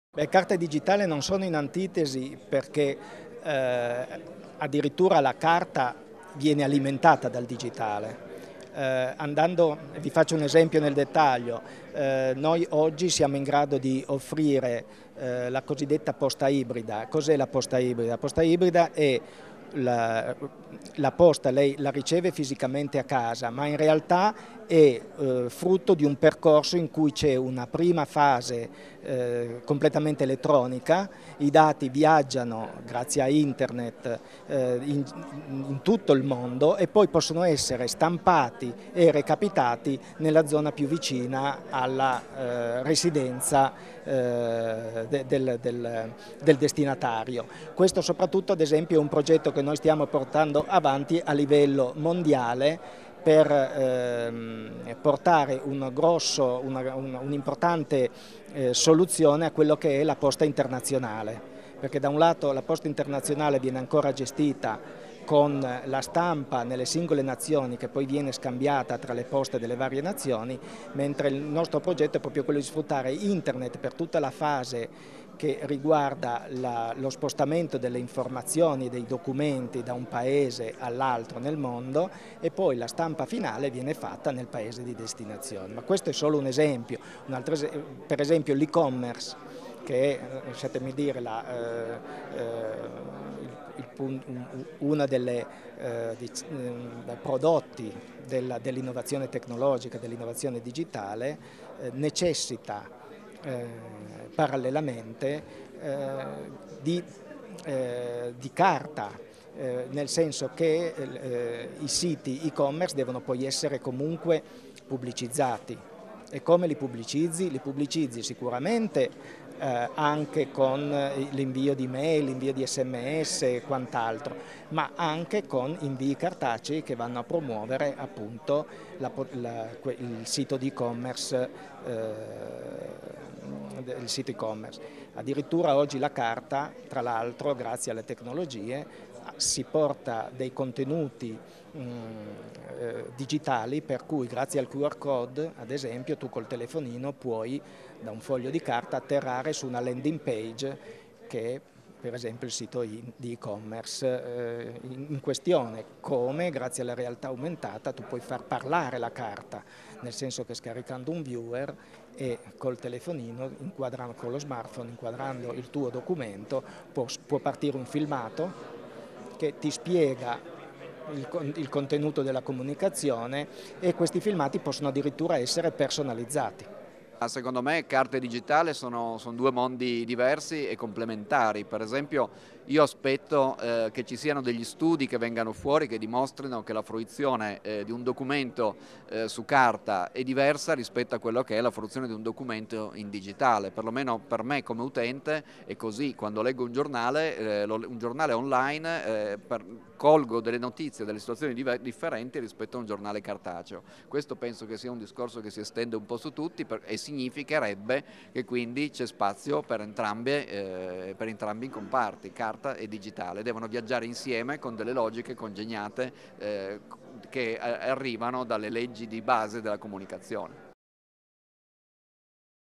CARTA E DIGITALE: QUALE IL RAPPORTO NEI TEMPI MODERNI?
L'utilizzo della carta ai giorni nostri, dove il digitale sta prendendo piede attraverso l'uso della rete. Se ne è parlato a Torino in un convegno organizzato dall'Unione Industriale, a pochi giorni dall'inaugurazione del Salone del Libro